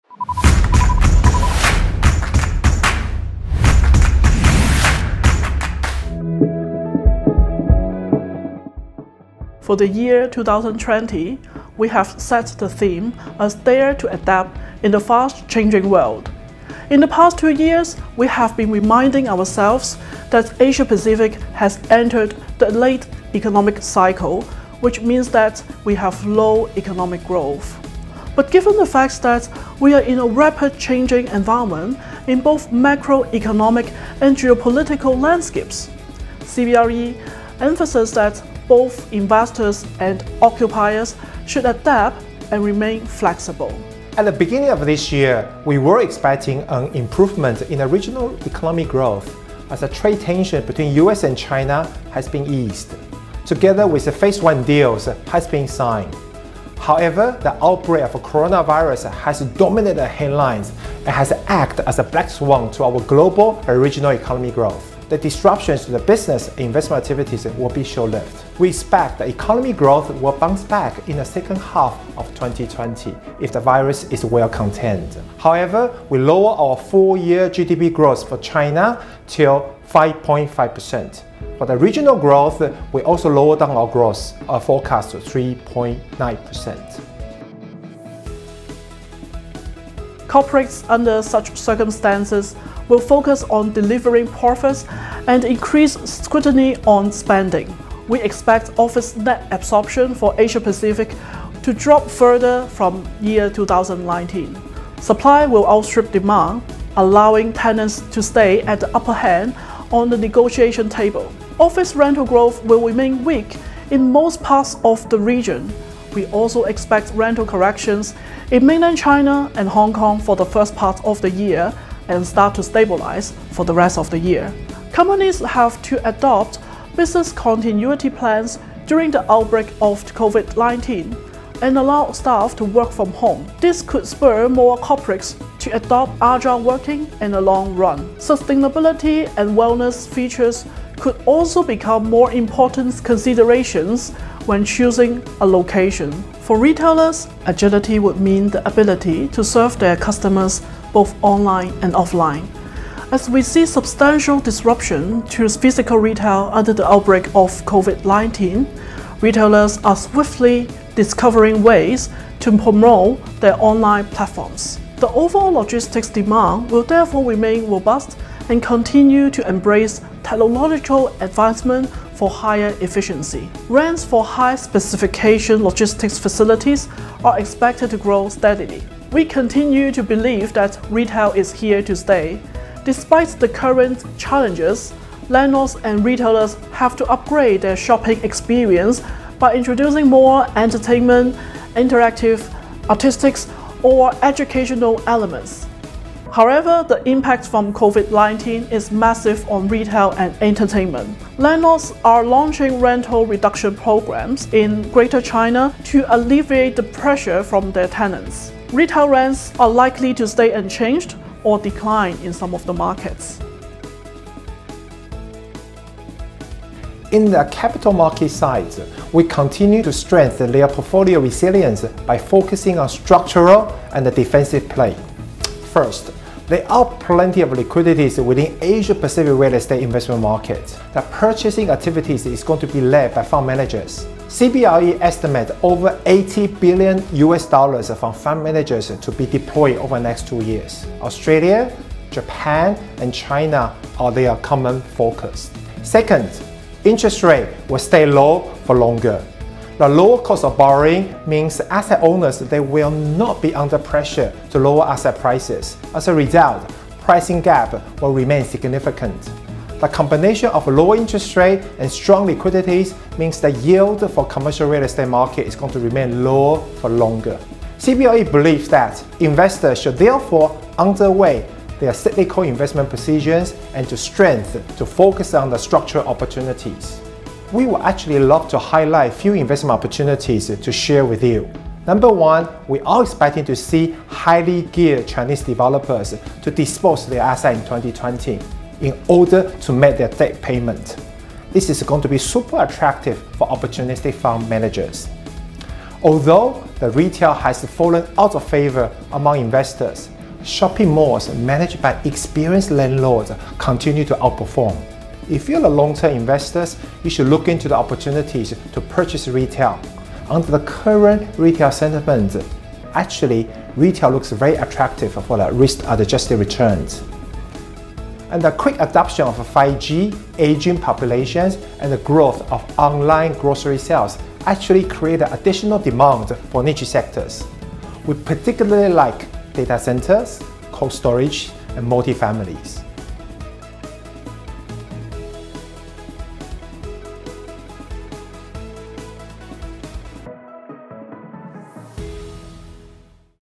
Asia Pacific Real Estate Market Outlook 2020
Against a rapidly changing environment in both economic and geo-political landscapes, property investors and occupiers should remain flexible. Where are the opportunities for real estate investors in 2020? For the full reports on both Singapore and Asia Pacific real estate market outlook,

3 key real estate investment opportunities:
• For opportunistic players, expect highly-geared Chinese developers to dispose their assets
• For long-term investors, look at retail assets for risk-adjusted returns
• For niche sectors, explore data centres, cold storage or multifamilies